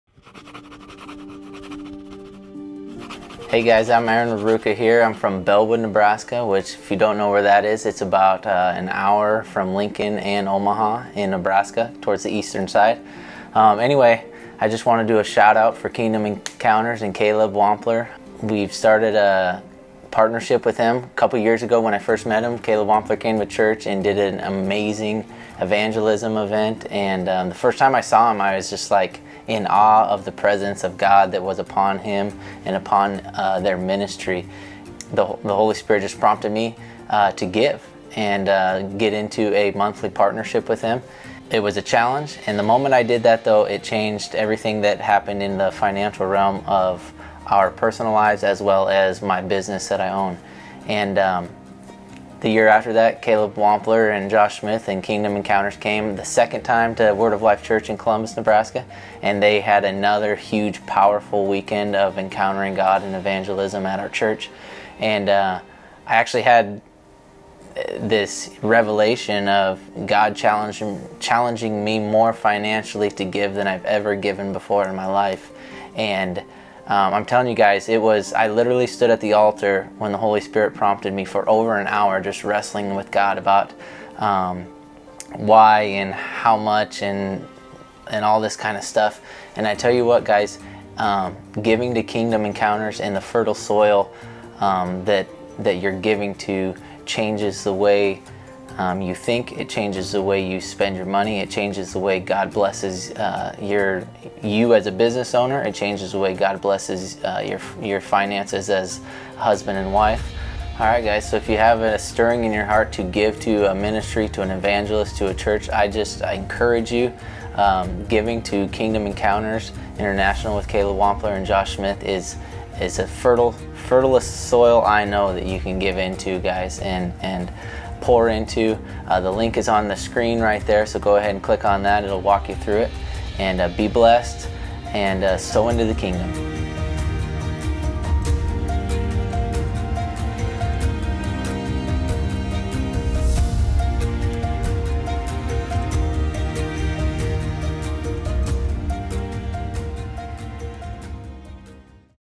Aaron's Testimony | Endorsement
Aaron Rerucha is one of our partners but it didn't start off that way. We came to their church a couple of years ago and the Lord moved mightily in the service. Aaron says he was in awe of the presence of God that was on Caleb, and the Lord started moving in his heart to become a monthly partner. God showed up in their personal finances and in their business in new ways. "Caleb, Joshua Smith, and Kingdom Encounters International came back to our church the following year, and again, the Lord gave me a revelation to give, but it to give more than I have ever given before in my life." Aaron wrestled with God at the altar for about an hour on why and how much to give. He listened and obeyed the prompting of the Holy Spirit and says that giving will change the way you think, the way you spend your money, it will bless your business, and it changes the way God blesses your finances. Kingdom Encounters International is fertile soil. If you have a stirring in your heart to give to a ministry, evangelist, or church, Kingdom Encounters International is good soil to give to.

To GIVE: kingdomencounters.us/give

Hunger Book Paperback:

Hunger Book Kindle

Hunger Audiobook: coming soon!